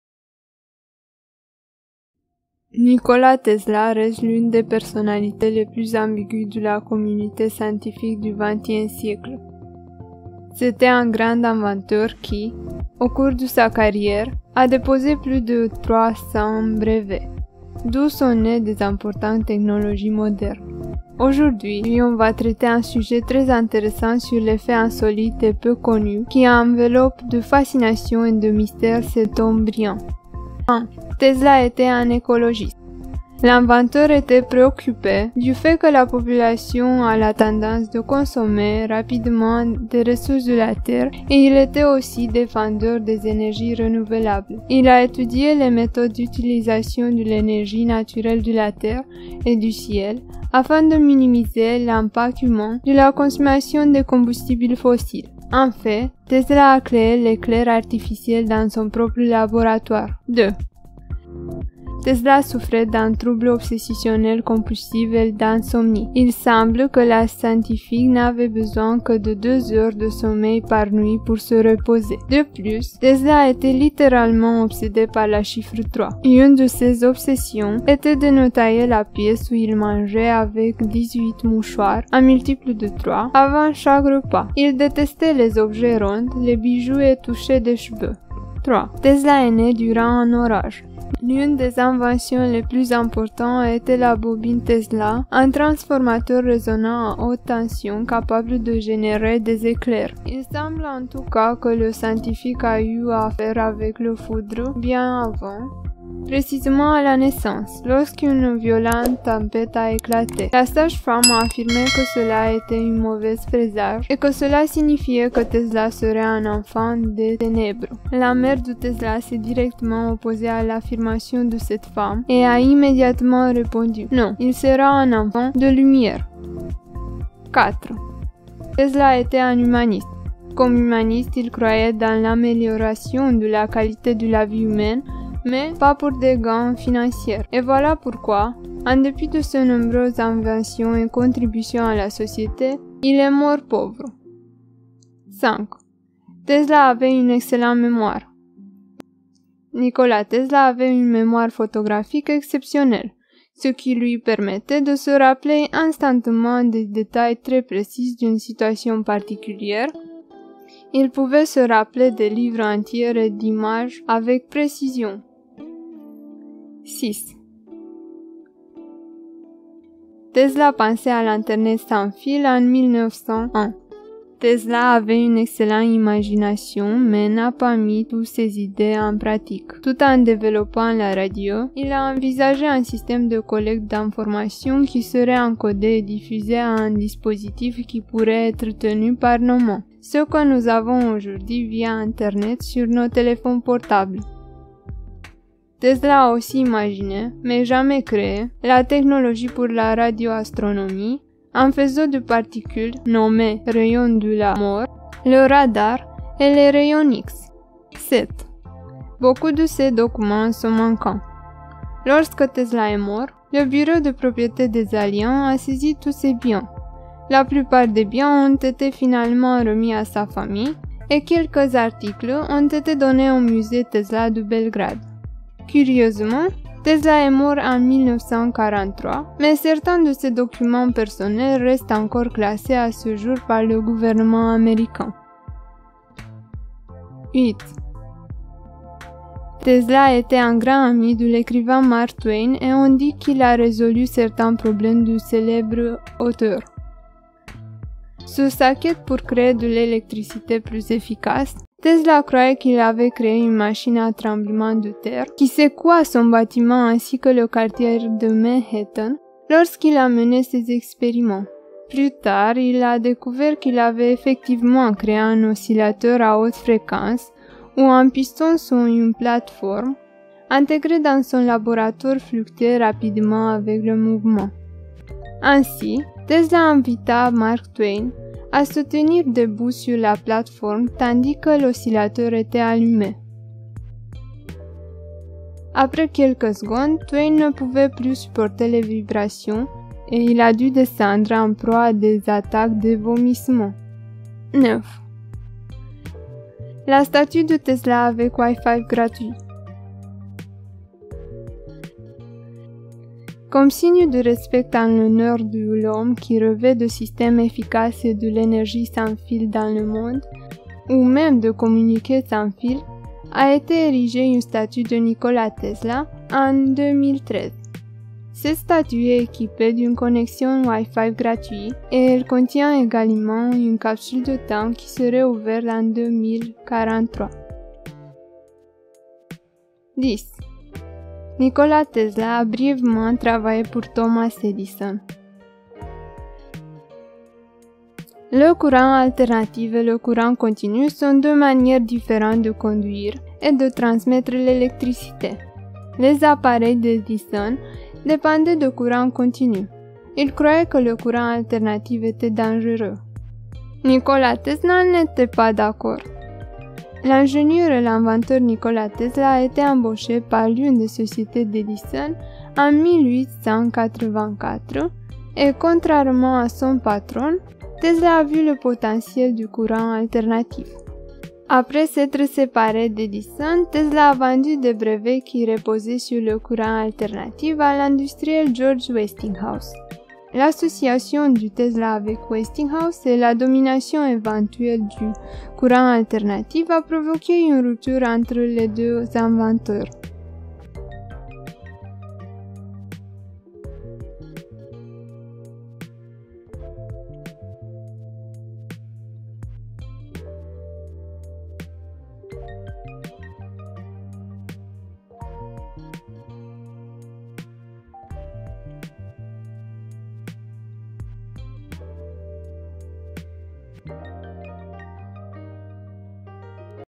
10 Curiosités Moins Connues Sur Nikola Tesla Qui Vont Vous Choquer, L'HOMME QUI A CHANGÉ LE MONDE
Les sous-titres sont disponibles dans toutes les langues. 💡 VOUS POUVEZ ACTIVER LES SOUS-TITRES en cliquant sur le texte [cc] en haut à droite.

 00:00 10 Curiosités Moins Connues Sur Nikola Tesla
 00:30 1.Tesla était un écologiste
 01:05 2.Tesla souffrait d'un trouble obsessionnel compulsif et d'insomnie.
 01:23 3.Tesla est né durant un orage
 02:05 4.Tesla était un humaniste
 02:25 5.Tesla avait une excellente mémoire.
 02:43 6.Tesla pensait à l’Internet sans fil
 03:26 7.Beaucoup de ses documents sont manquants.
 03:55 8.Tesla était un grand ami de l'écrivain Mark Twain
 04:50 9.La statue de Tesla avec Wi-Fi gratuit.
 05:24 10.Nikola Tesla a brièvement travaillé pour Thomas Edison.

Tesla, une vie de labeur.
Les inventions de Nikola Tesla, l'homme qui a inventé le XXe siècle ⚡️
Nikola Tesla's Mysterious Electricity Generator | The Complete Physics
La grande révélation : Nikola Tesla l'histoire de l'énergie gratuite ou libre